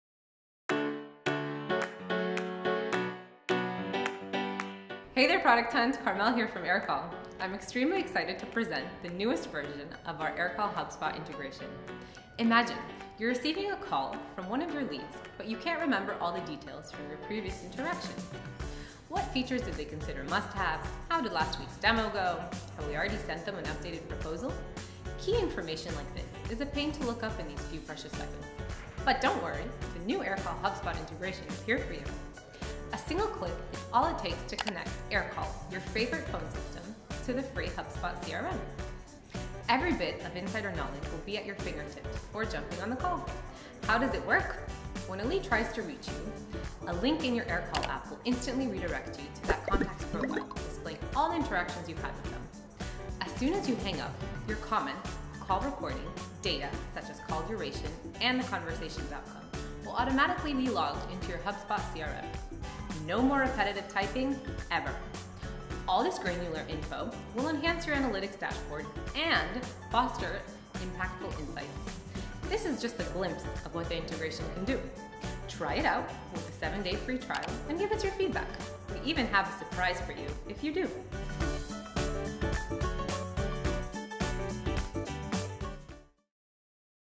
The Aircall for HubSpot Integration
All it takes is one click to connect your free HubSpot CRM with Aircall to have all the information you need before jumping on a call.